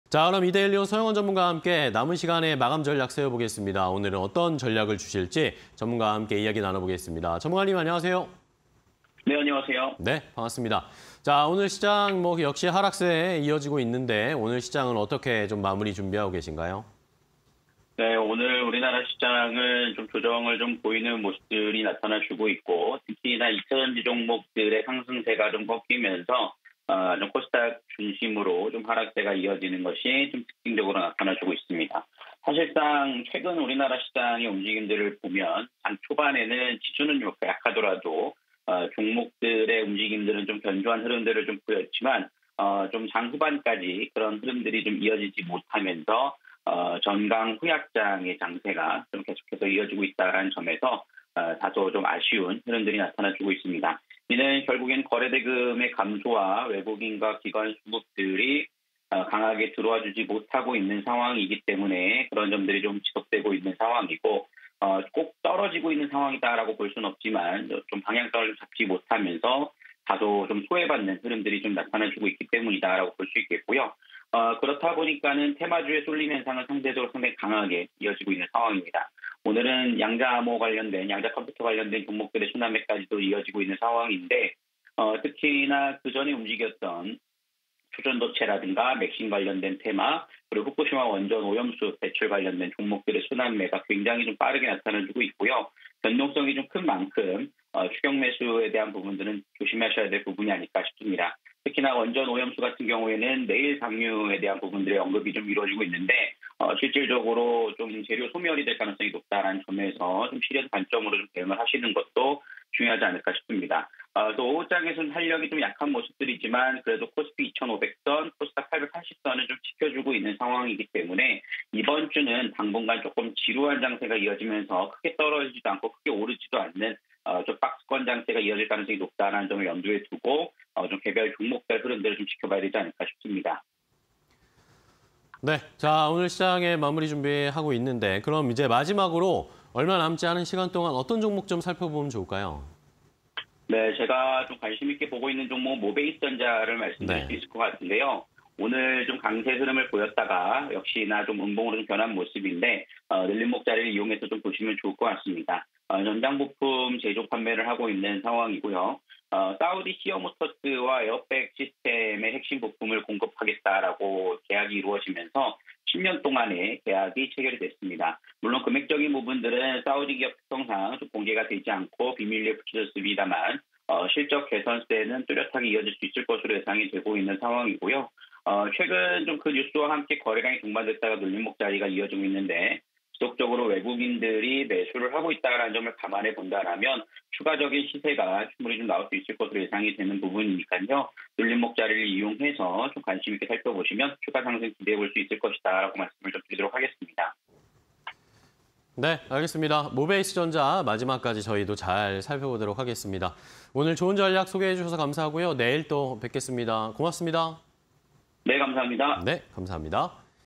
모베이스전자(012860)... 사우디 '시어모터스'와 핵심 부품 공급 계약_서용원 마감 전략 (20230823)
- 국내 증시, 2차전지 섹터 조정으로 약세
- 테마주 순환매 지속… '폭탄 돌리기' 주의
- 코스피 2,500선ㆍ코스닥 860선 지지 확인

모베이스전자(012860)
- 자동차 전자ㆍ전장부품 제조 및 판매
- 사우디 '시어모터스'와 핵심 부품 공급 계약
- 실적 성장세 확대